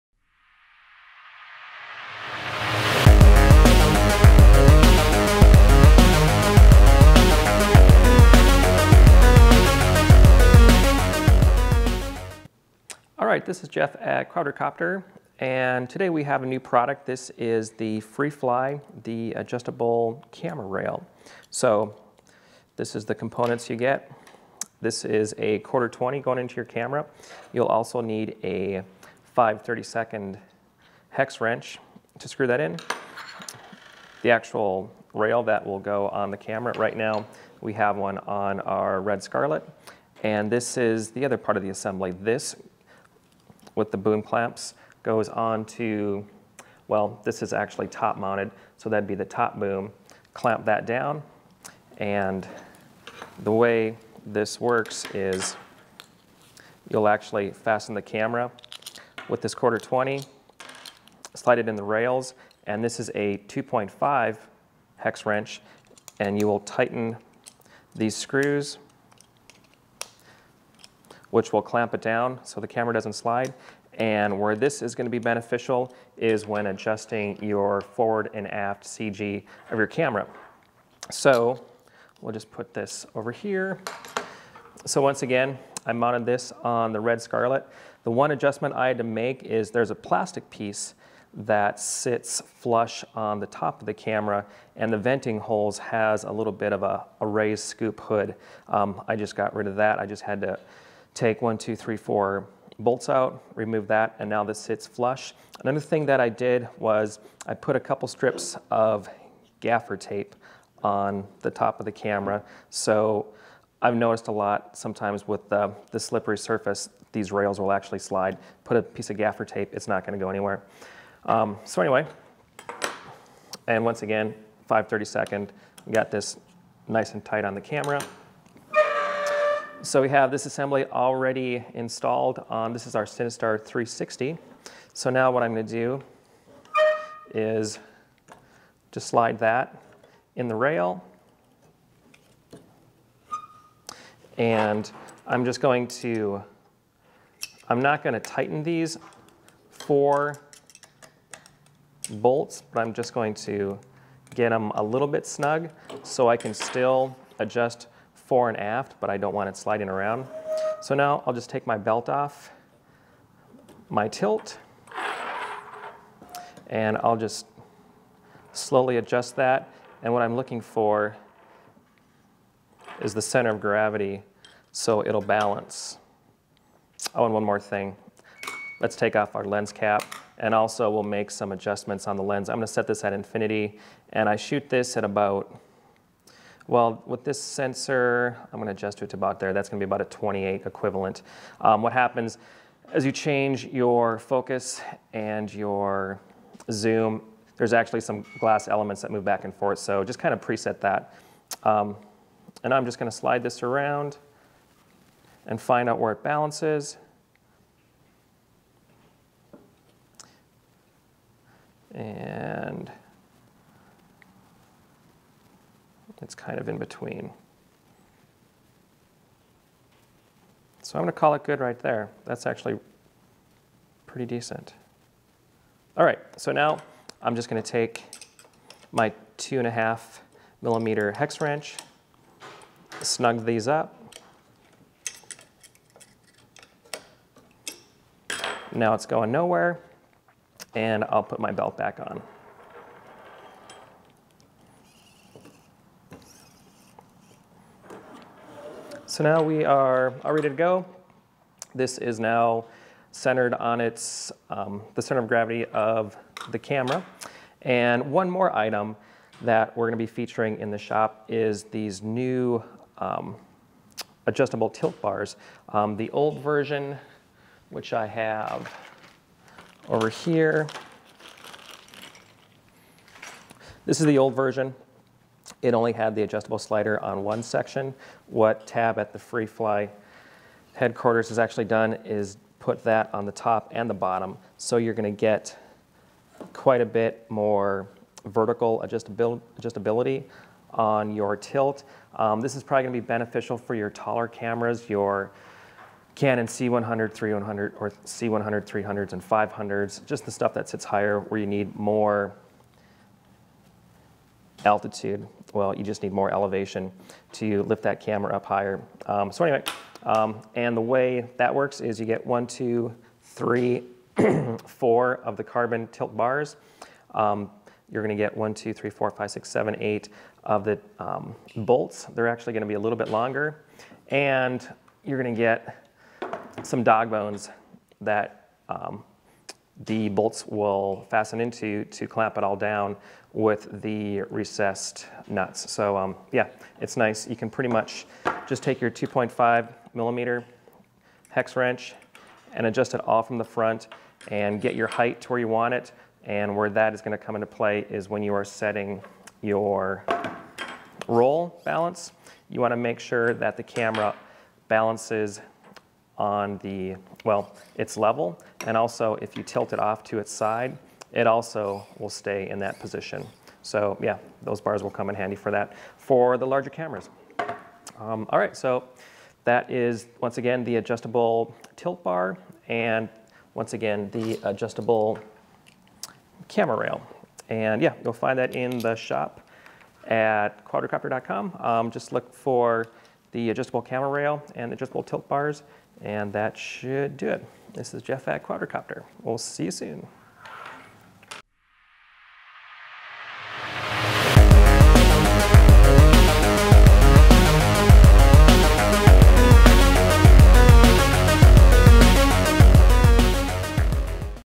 Quadrocopter Tutorial - Adjustable Camera Rail and Tilt Bars
Jeff describes the operation of the new Freefly adjustable camera rail and tilt bars.